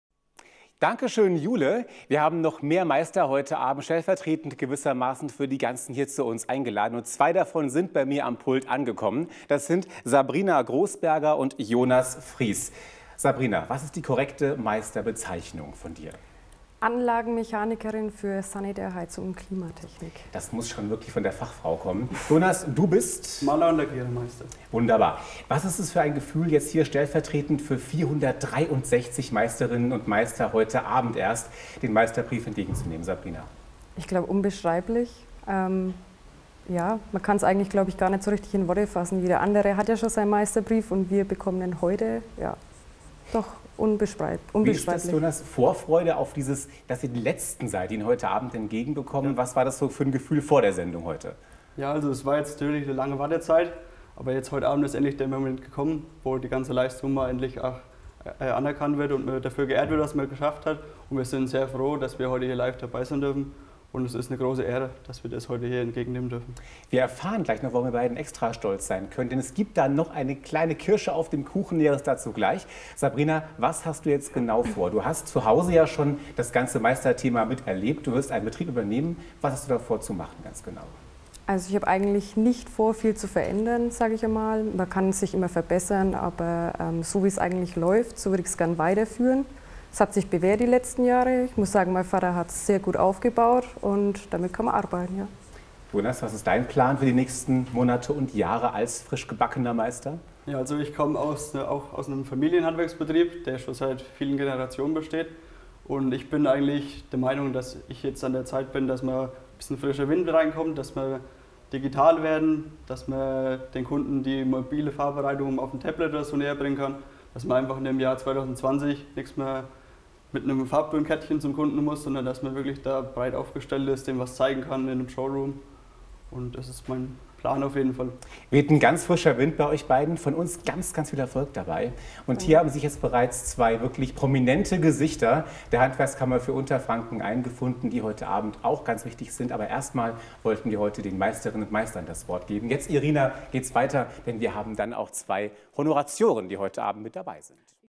Highlights Meisterfeier 2020: Studiointerview
Sabrina Großberger, Installateur- und Heizungsbauermeisterin aus Obernzenn, und Jonas Fries, Maler- und Lackierermeister aus Bischofsheim v. d. Rhön, im Interview mit Daniel Pesch von TV Mainfranken.